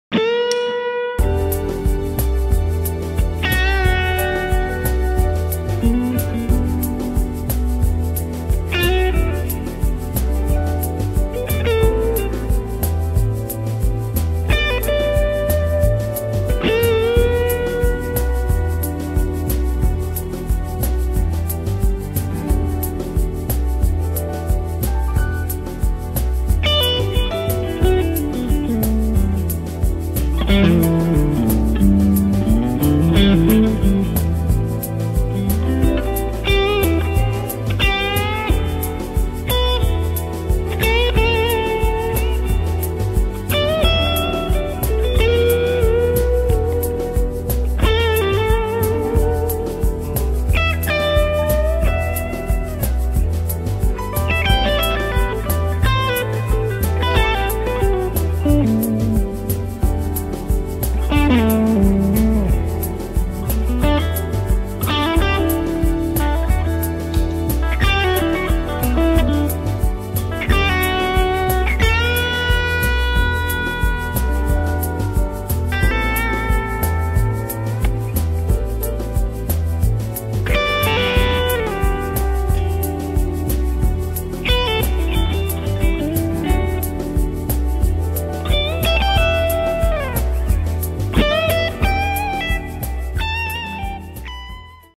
2016 Nik Huber Krautster II goldtop, Part2
clean & 2016 Okko Diablo Gain+ "Boostmonster" Overdrive LTD Edition / Amp: Fender Vibroverb (original 1963), Vahlbruch Spacetime Delay, TC Polytune Tuner, ChickenPick Picks